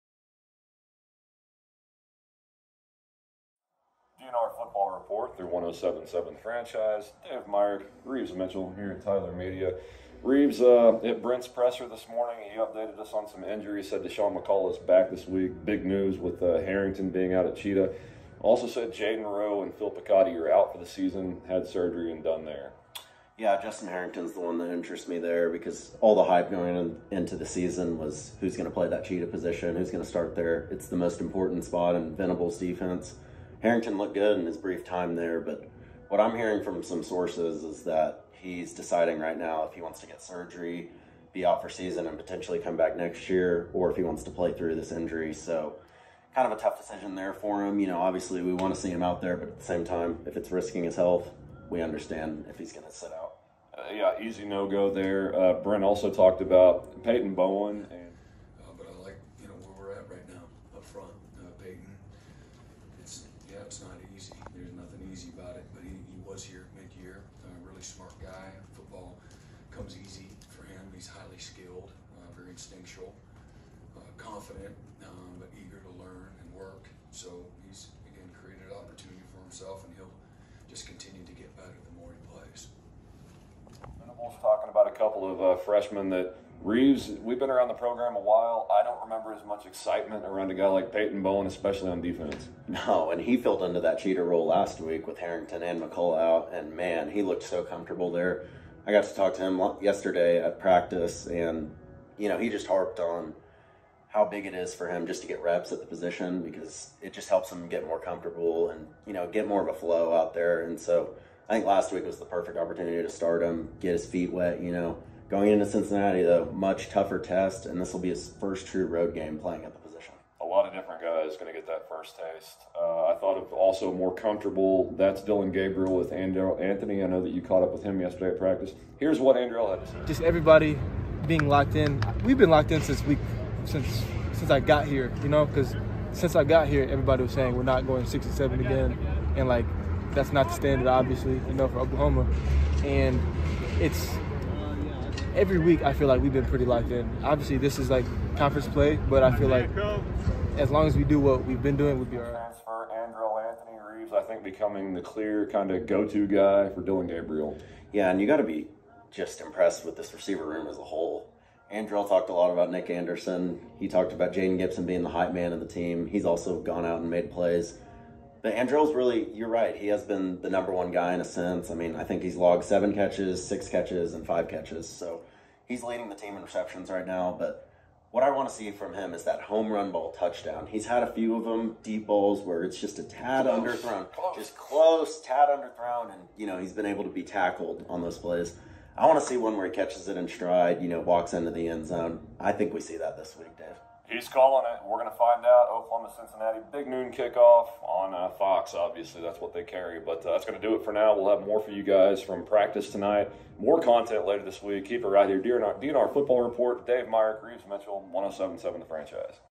D&R OU Practice Report - Wednesday, September 20
Dave and Rives are back to give you the latest on OU player interviews and practice this week. They also look ahead to OU’s conference opener in Cincy this weekend.